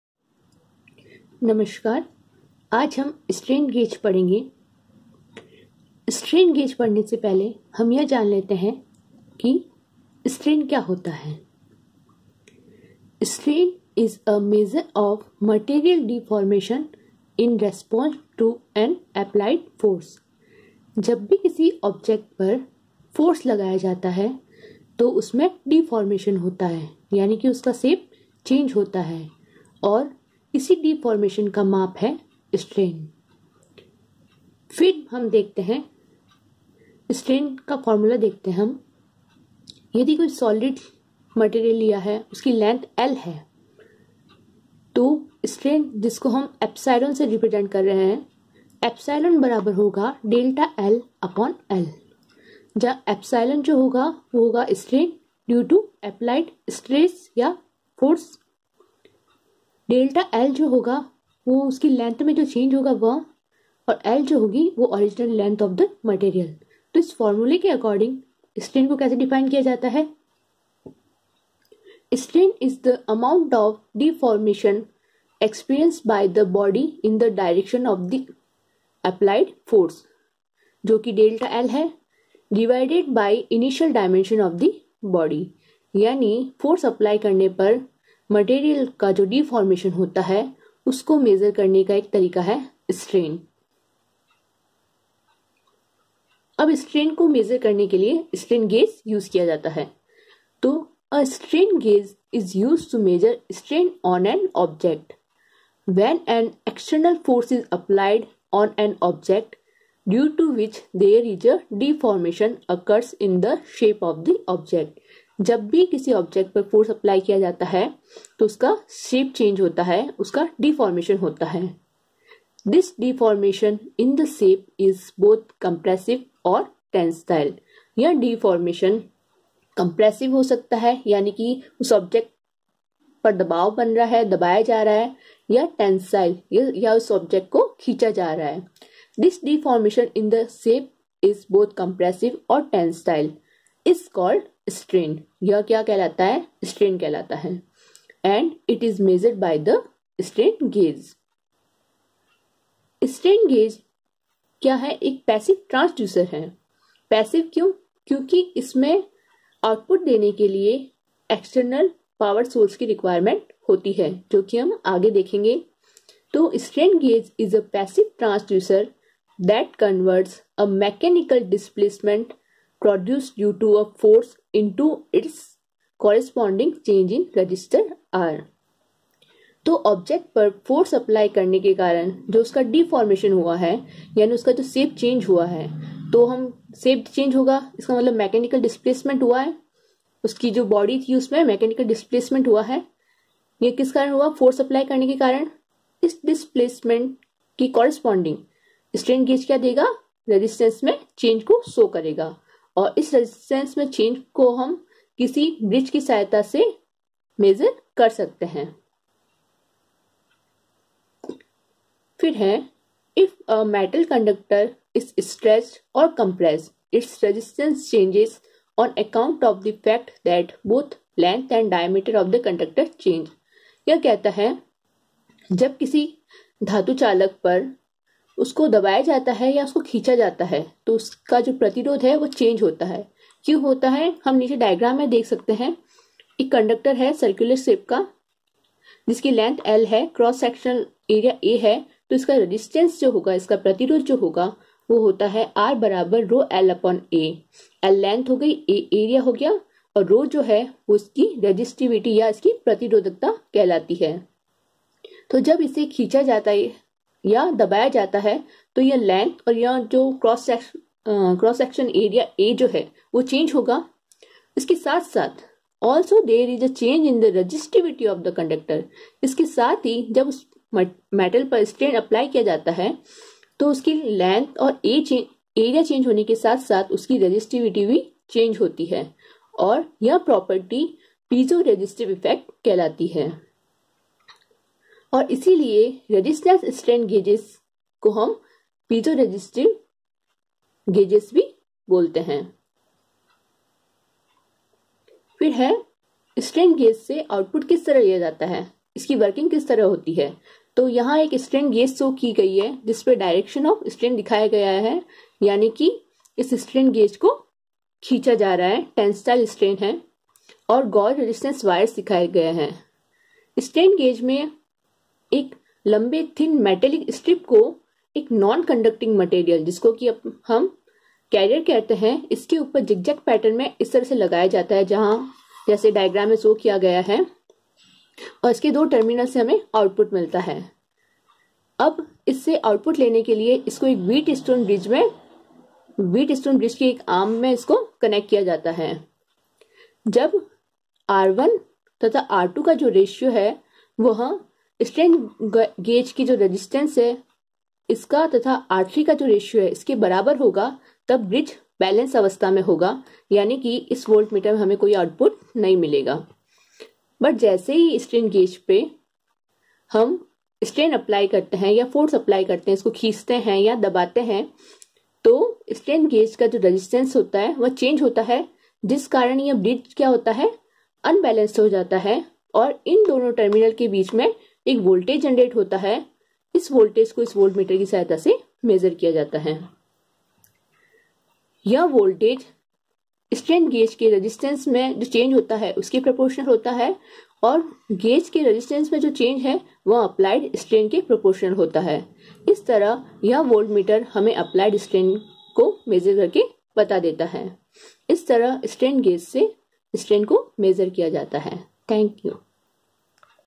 EL 203 Unit 2 - Strain guage Part-1 (Chandani Sankhala, GPC Jalore)
This e Lecture is in accordance with syllabus of Board of Technical Education, Rajasthan